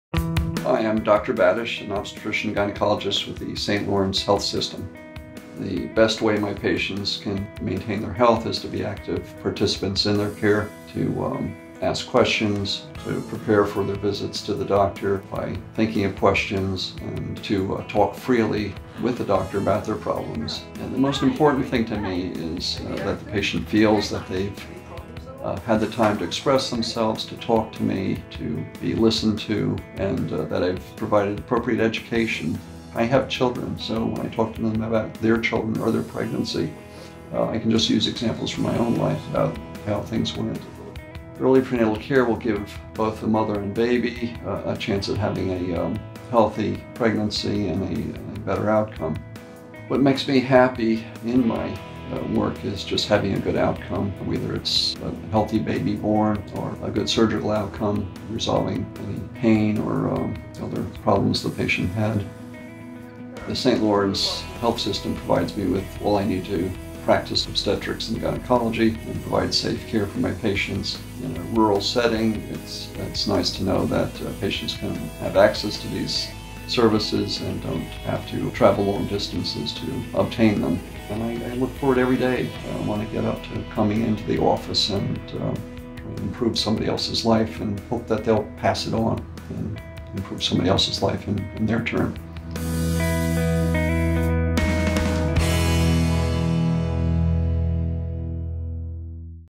St. Lawrence Health System | OBGYN Dr. John Batich
The St. Lawrence Health System and Canton-Potsdam Hospital welcome Dr. John Batich to its medical team.